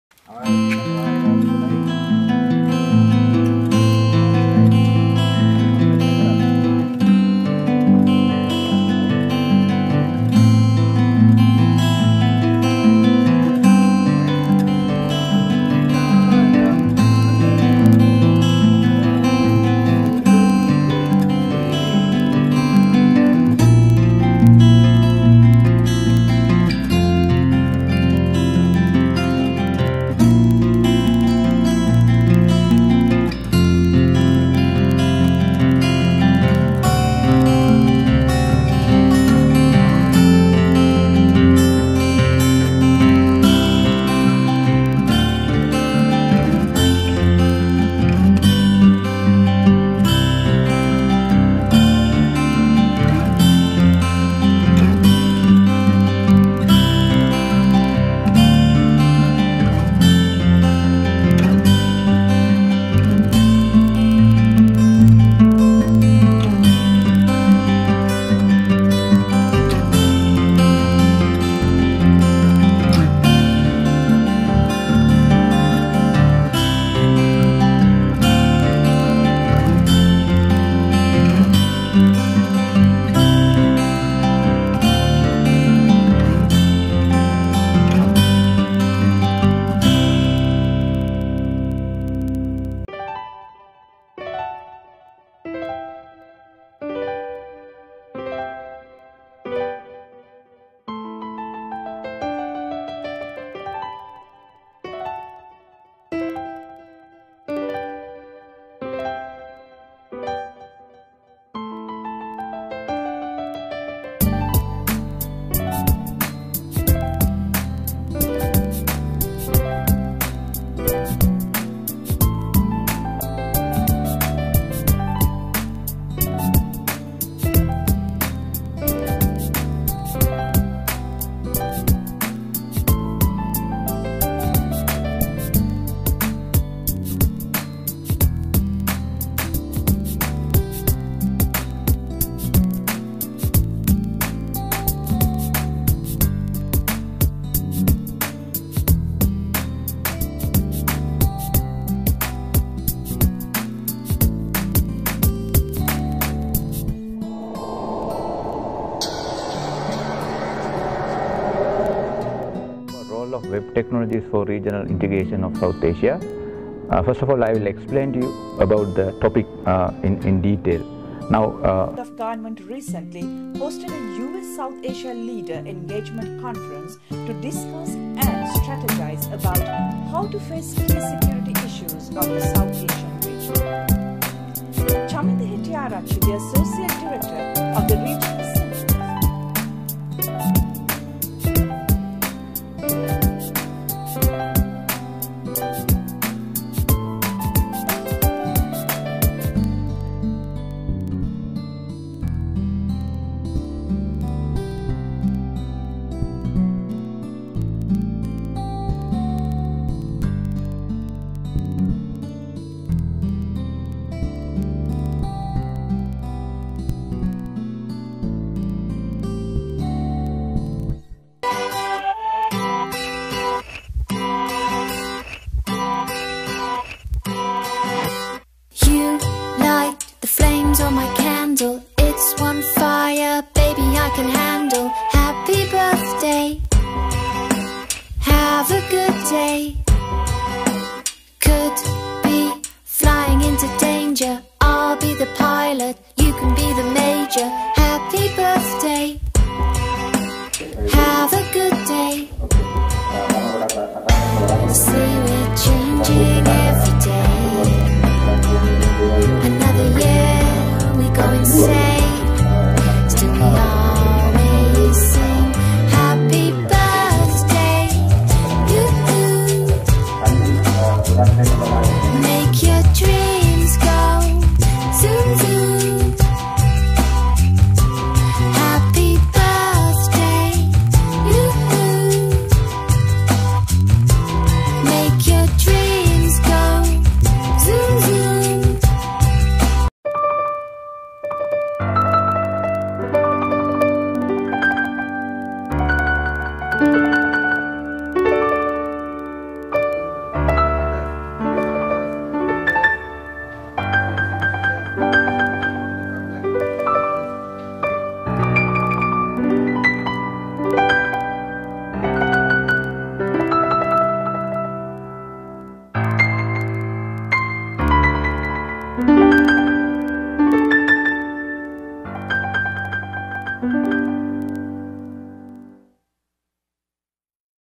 Birthday Greetings for Chaminda Hettiarachchi on 3rd January, 2016
This video has been created using Chaminda Hettiarachchi's Facebook pictures and YouTube videos  to wish him on his birthday on 3rd January, 2015 by a group of friends and fans.